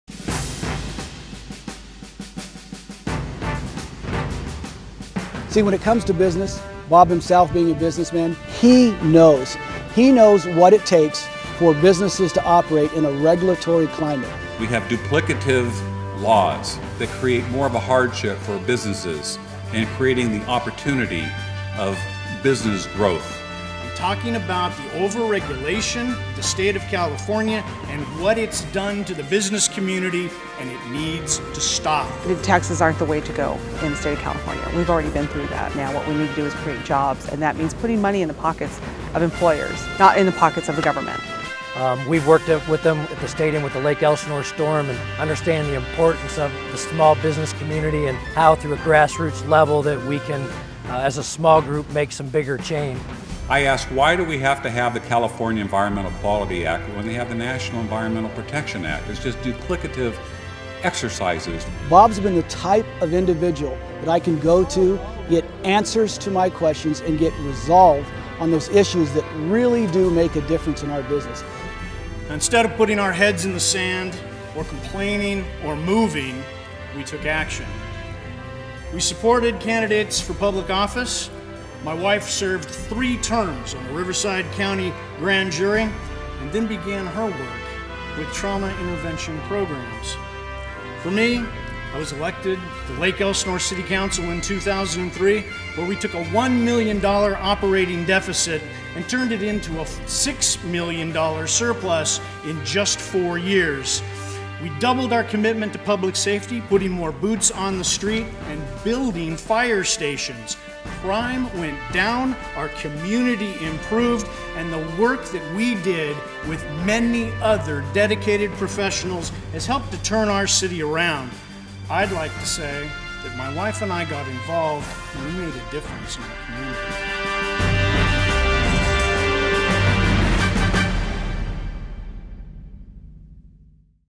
Will Bob Magee's Experience as a businessman and elected official help?
Bob Magee, City Councilmember of Lake Elsinore, is running for the newly formed 67th California State Assembly District.  This new district is largely represented by Assemblyman Kevin Jeffries who is unable to run again due to term limits.

Portions of the new District are also currently represented by Assemblymembers Cook & Nestande.   All of theses Assemblymembers have endorsed Magee's campaign.

Magee has secured over 100 endorsements from current and former elected and appointed officials primarily from Riverside County. This massive show of in-district support has solidified his grass roots base.

Remember to Vote Bob Magee on June 5th, 2012!